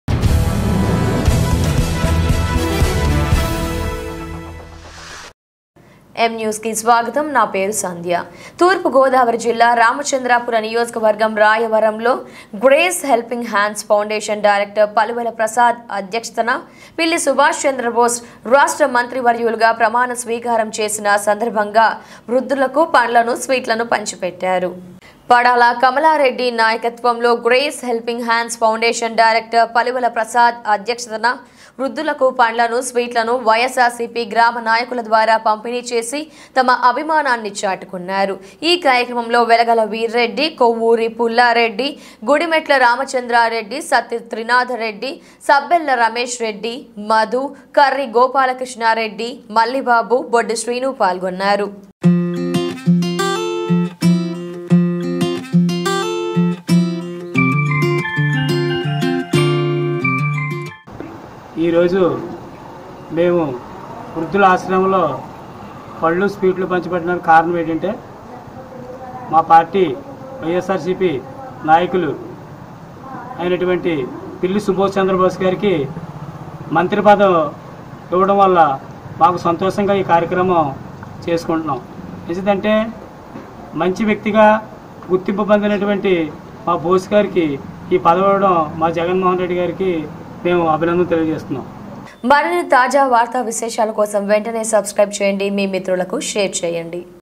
పిల్లి సుభాష్ చంద్రబోస్ రాష్ట్ర మంత్రి | వృద్దులకు పండ్లు, స్వీట్లు పండ్లు || M NEWS TELUGU
పిల్లి సుభాష్ చంద్రబోస్ రాష్ట్ర మంత్రివర్యూలుగా ప్రమాణ స్వీకారం | వృద్దులకు పండ్లు, స్వీట్లు పండ్లు || M NEWS TELUGU